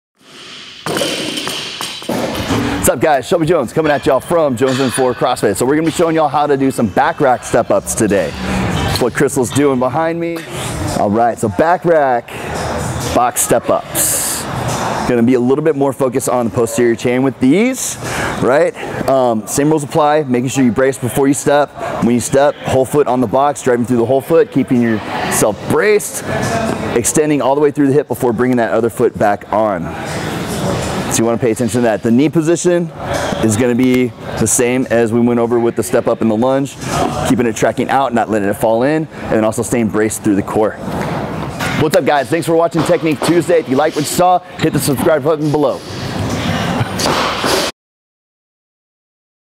How to do Back Rack Step Ups | Technique Tuesdays | Jonesn4CrossFit | San Antonio, Texas
Welcome to Technique Tuesday with Shelby Jones!

We’ve been talking about beginner step ups & box step ups, but now we want to show you what to do when you’re ready to move to the next level!

Adding an outside stimulus like a dumbbell, kettlebell or barbell can really help to make this exercise a bit more of a challenge.

The back rack step up is a more advance version of the box step up, utilizing additional weight.

To perform this exercise, you are going to take a rack and place it gently on your back shoulders. Throughout the movement, you are going to focus on the posterior chain, keeping it tight. Then brace before you step & place your whole foot on the box. As you rise, exten all the way through the hip, bracing the core & having the knee position tracking out.

As always, if you guys have any questions on this exercise, drop them in the comments below!